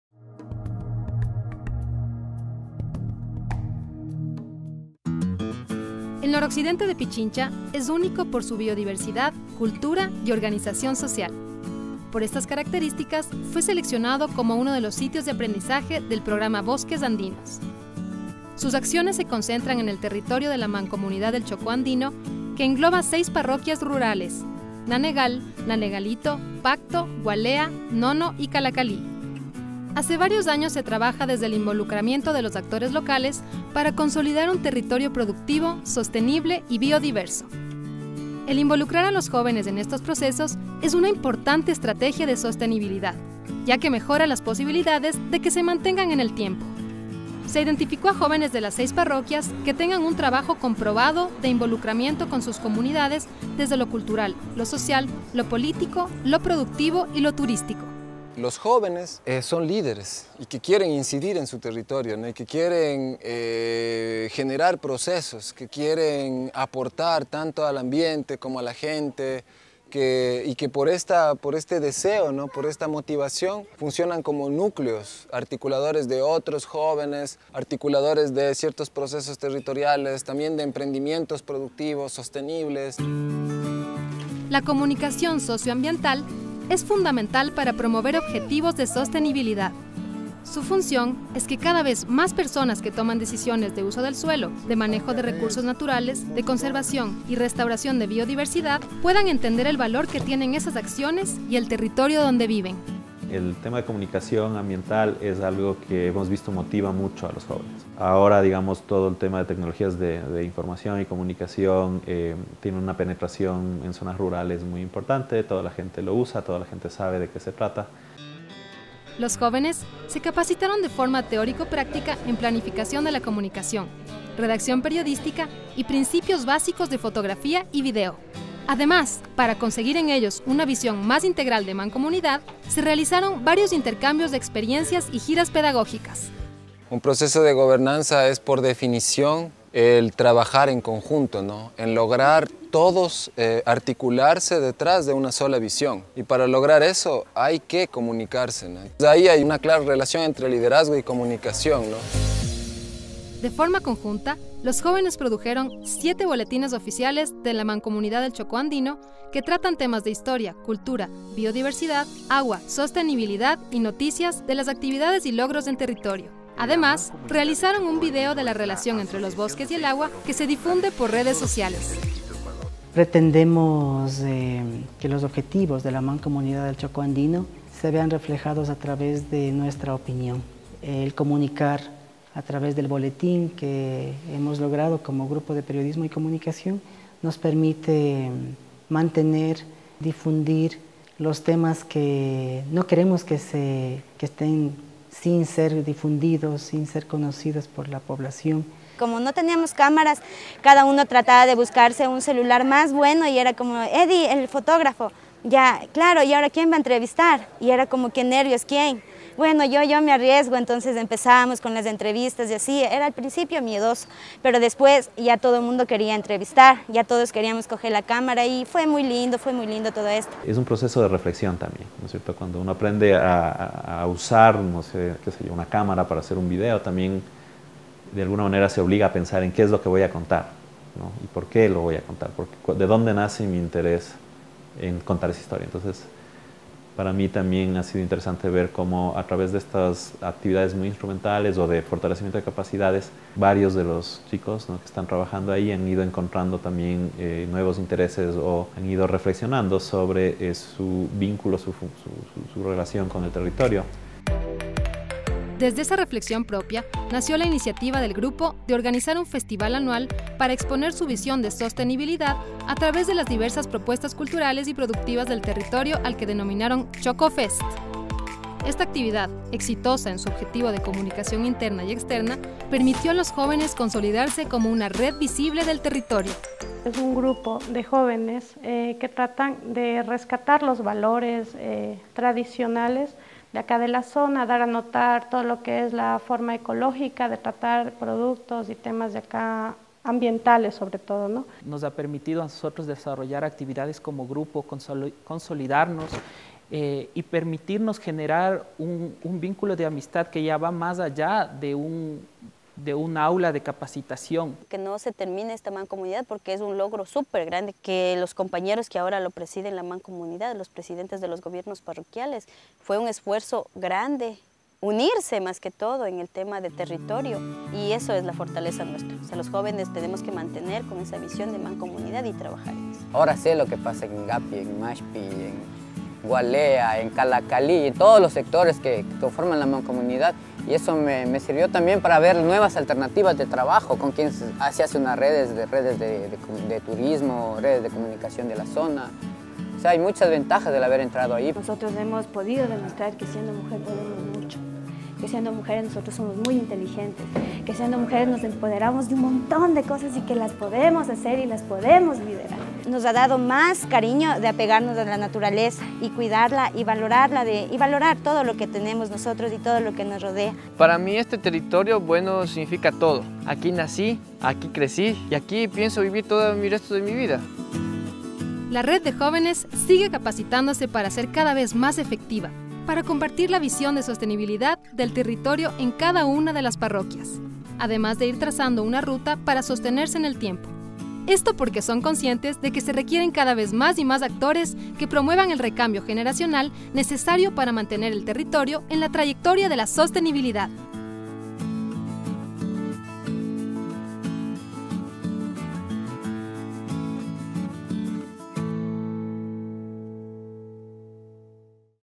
Red de Jóvenes del Chocó Andino: transformando lo rural desde la reflexión, acción y comunicación
La Red de Jóvenes del Chocó Andino es un conglomerado de jóvenes rurales en constante aprendizaje y crecimiento que lideran procesos endógenos de sostenibilidad a favor del Chocó Andino, utilizando como principal herramienta la comunicación socio – ambiental. El video muestra el desarrollo de su proceso de reflexión, conformación, fortalecimiento y acción.